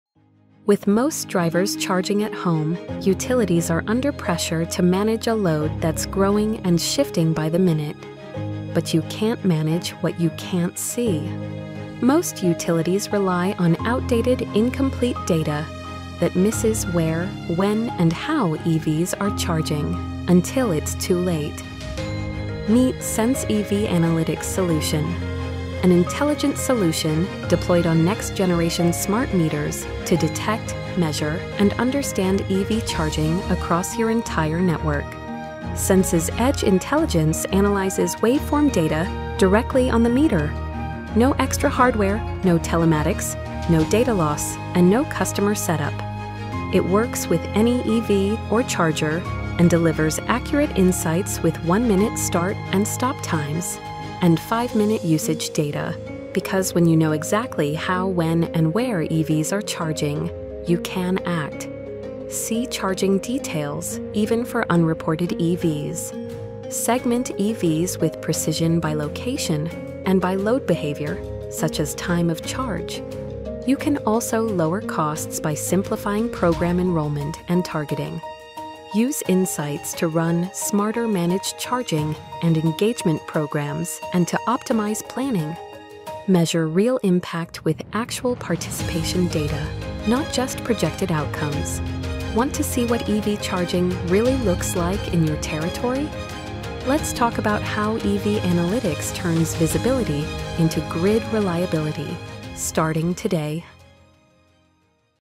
Introducing Sense EV Analytics
Smarter EV Insights. Greater Grid Reliability.
As EV adoption accelerates, utilities need reliable data for distribution system planning and implementing EV charging programs that work for customers. But today’s solutions are often expensive, complex, and riddled with data gaps—creating more questions than answers.

Sense EV Analytics changes that. Using high-resolution waveform data on the meter, we detect EV presence in the home and capture minute-level charging behaviors—including both Level 1 and Level 2 events. That means real-time visibility, accurate planning, and better service for your customers And because our solution is car- and charger-agnostic, it works across your entire residential service  territory—without adding hardware or complexity.

Fewer blind spots. Lower costs. More opportunities to scale.